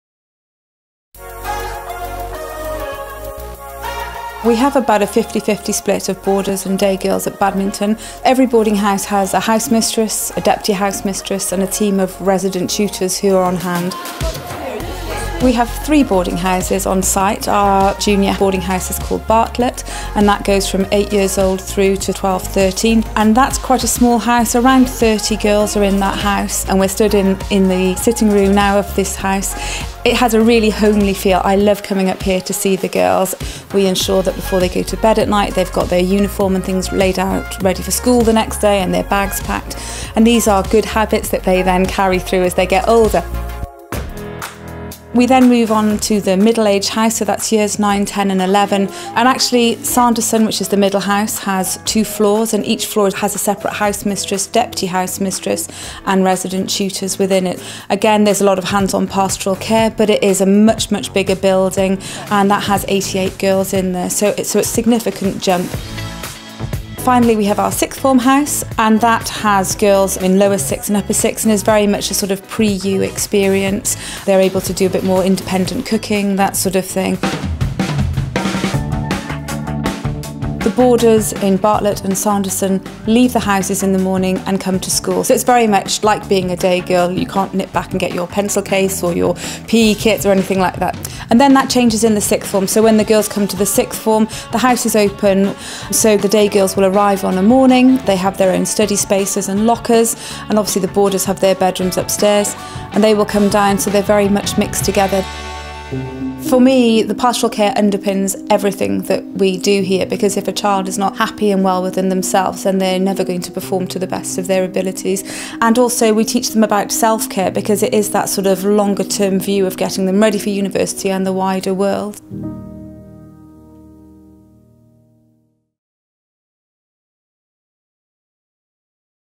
Welcome to Boarding at Badminton
An introduction to Boarding at Badminton. Find out more about what it's like to live here.